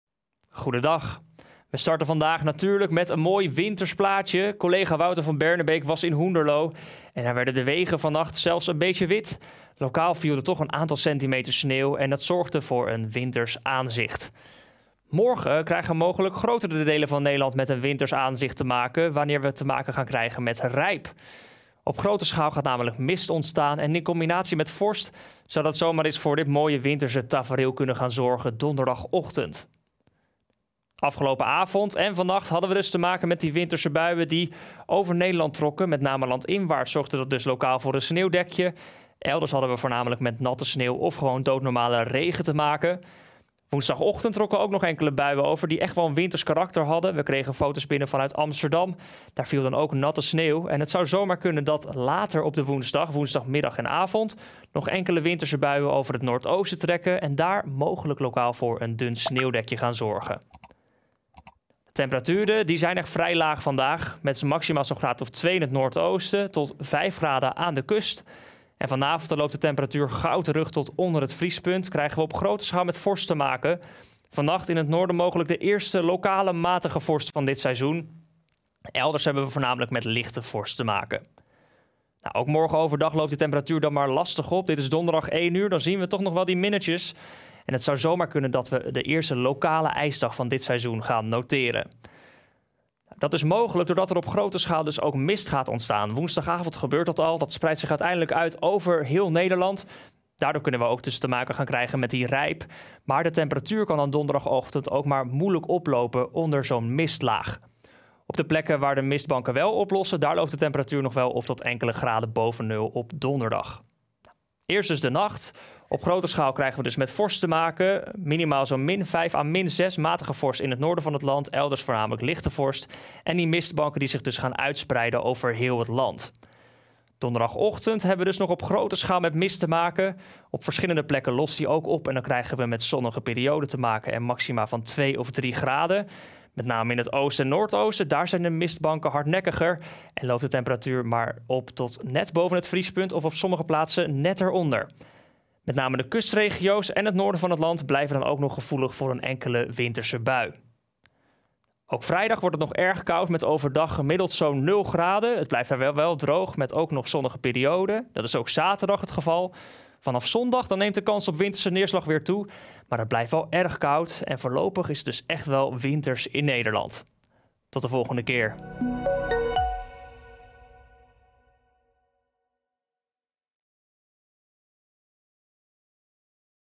Weerbericht: enkele winterse buien, daarna matige vorst?
Regionaal kwam het afgelopen dinsdagavond en nacht tot een sneeuwdek. Kunnen we nog meer winters weer verwachten? Donny de Koning vertelt je er meer over!